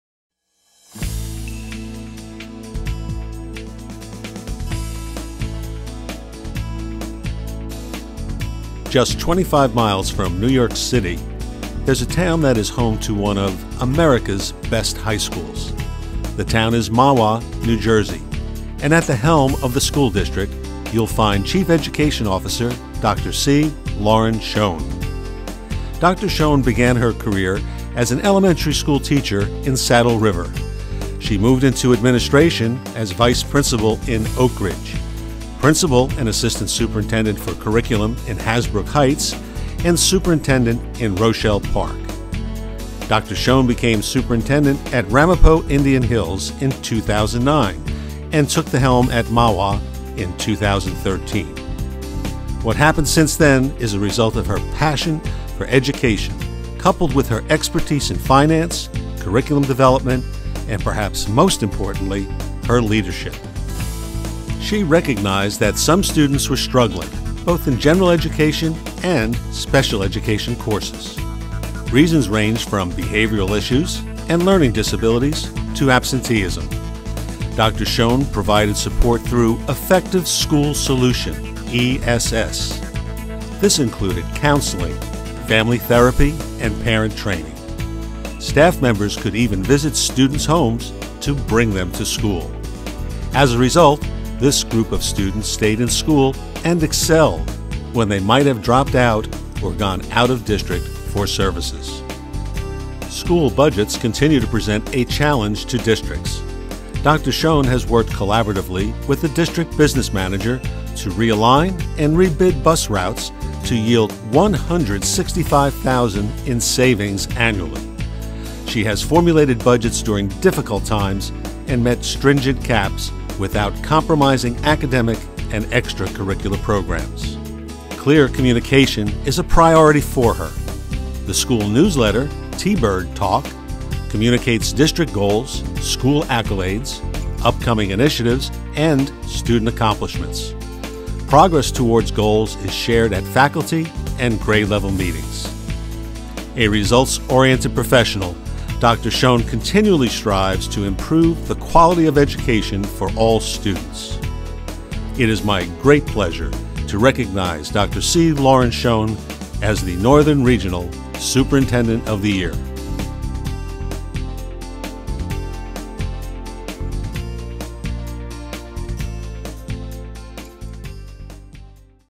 Dr. C. Lauren Schoen Superintendent of the Year from Mahwah School District Closes Education Gap
Dr. C. Lauren Schoen, Chief Education Officer for the Mahwah School District, is NJASA's Northern Regional Superintendent of the Year.  Her visionary leadership assisted in the implementation of the Effective Schools Solution’s (ESS) program at the Mahwah Public School District and the Ramapo Indian Hills Regional High School District. This provided a significant percentage of general education and special education students with opportunities to remain in-district while improving and enhancing their educational experiences.  Watch our video to learn more.